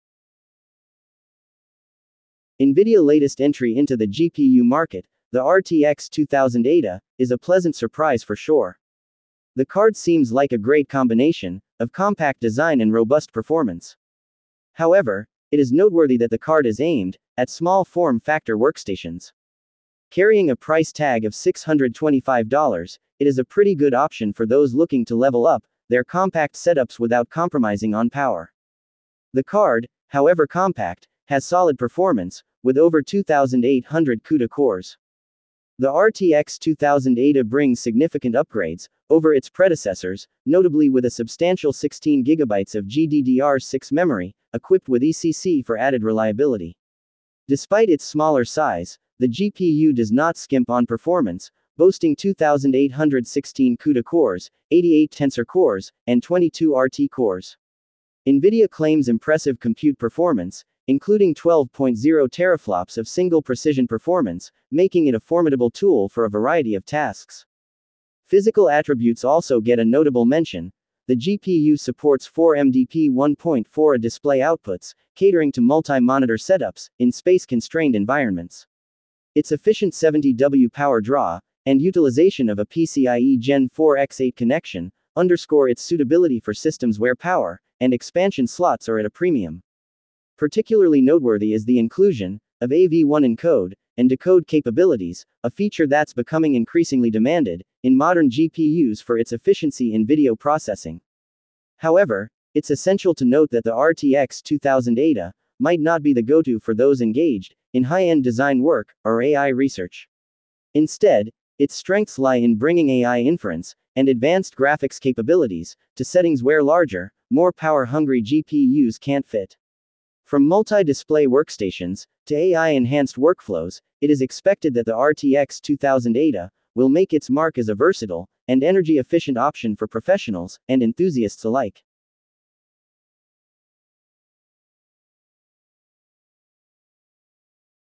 NVIDIA RTX 2000 Ada Workstations GPU: First Look - Reviews Full Specifications
NVIDIA‘s latest entry into the GPU market, the RTX 2000 Ada, is a pleasant surprise for sure. The card seems like a great combination of compact design and robust performance. However, it is noteworthy that the card is aimed at small form factor workstations. Carrying a price tag of $625, it is a pretty good option for those looking to level up their compact setups without compromising on power.